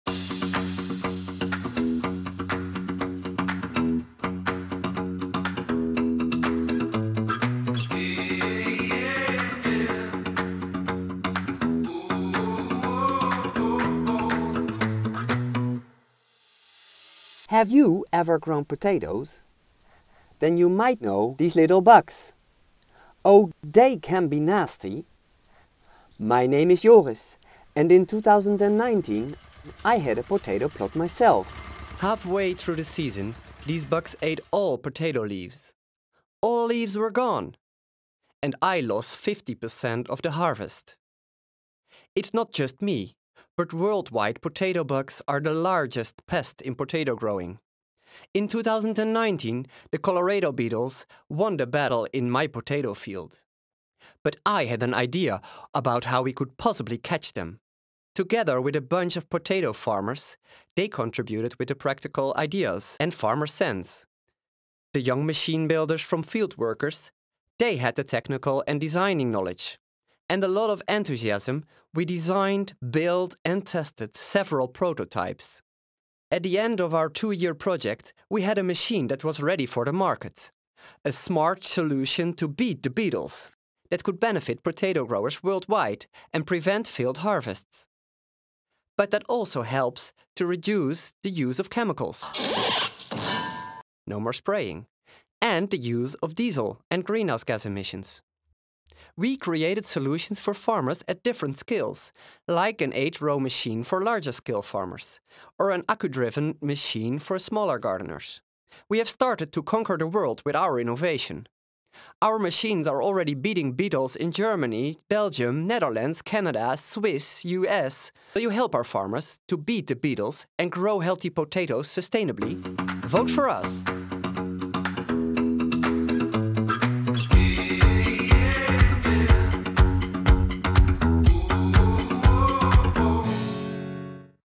Operational Group: Colorado Beetle Catcher - Sustainable Machine Pest Control
Operational Group: Colorado Beetle Catcher -Sustainable Machine Pest Control (the Netherlands)

This Operational Group won the EIP-AGRI Innovation Award 2024 for the category ‘Sustainable management of natural resources in farming practices’.

Hotter summers are increasing the occurrence of the Colorado potato beetle on Dutch farms. Farmers, designers, technicians and experts worked together in this Operational Group from the Netherlands to develop and test a mechanical pest control machine. It takes advantage of the natural behaviour of the Colorado beetle, catching them in a selective and sustainable manner. The use of this machine reduces the need to chemically combat this pest. An environmental subsidy is given to Dutch farmers to purchase this machine and more than 20 farms have already placed orders. Over 50 on-farm demonstrations are scheduled across Europe next year. In addition, an adapted version is now being used in France against the weevil beetle in alfalfa production.